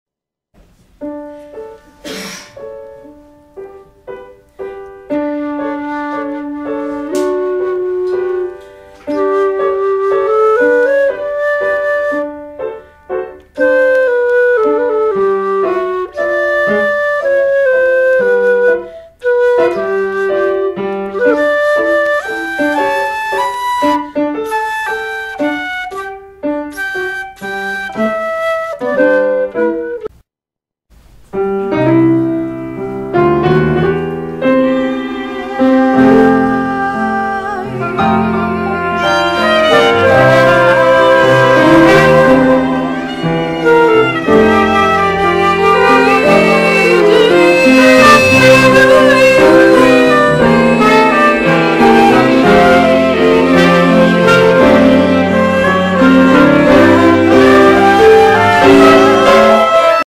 Open Improvisations
Free-style improvisation with Jazz Improvisation class at the University of Toronto with Phil Nimmons !  I am performing on Flute, I take no credit for the other instrumentalists.